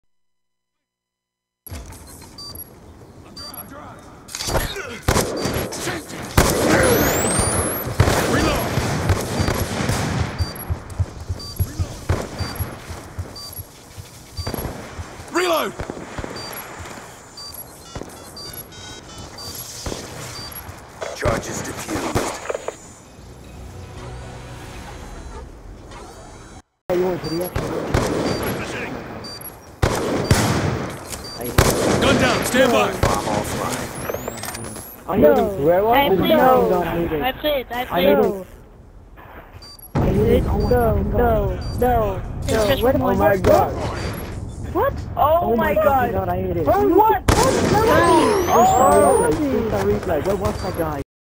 Rease_Brutal | Insane 1440 wallbang ladderstall
Heey guys, as you can see i play on me Rease account again, thats because i wanna bring out 2 episodes before joining any other clans. I hit this shot on my second game that day, and it was like so Random, i had no clue where that guy was... looking it back and i saw he was in the middle, in front of the tower xD, also ReaseUpsurge (My clan) is going to Upload his Promo tonight, so be sure to check that out.
I'm out!